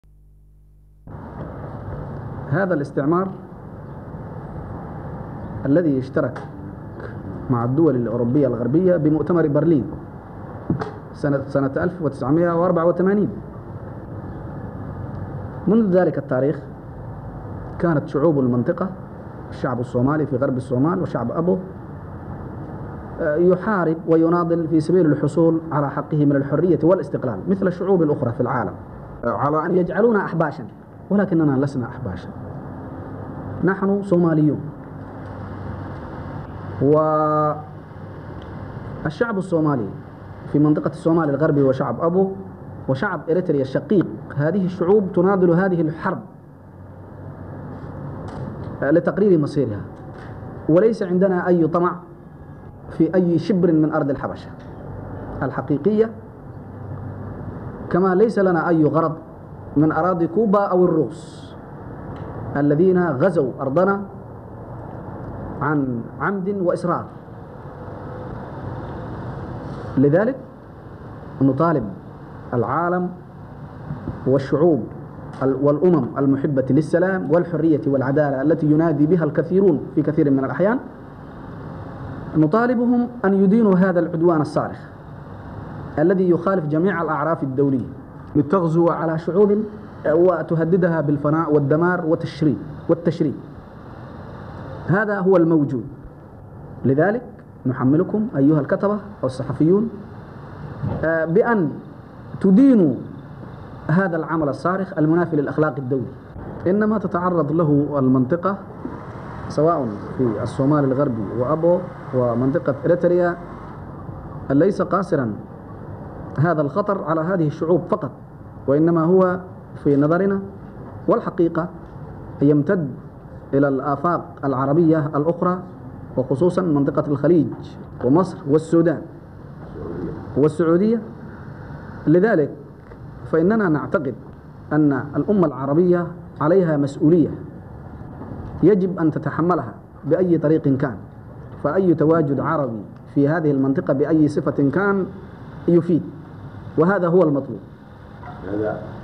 SYND 18 2 78 SECRETART GENERAL OF WEST SOMALI LIBERATION FRONT MAHMOUD GIVES STATEMENT
(18 Feb 1978) The Secretary General of the Western Somali Liberation Front Mr Abdallah Hassan Mahmoud gives a press conference in Rabat, Morocco. He appeals for military and medical assistance and tells newsmen that there are now  ten thousand soviet troops involved in the Ogaden conflict in Ethiopia.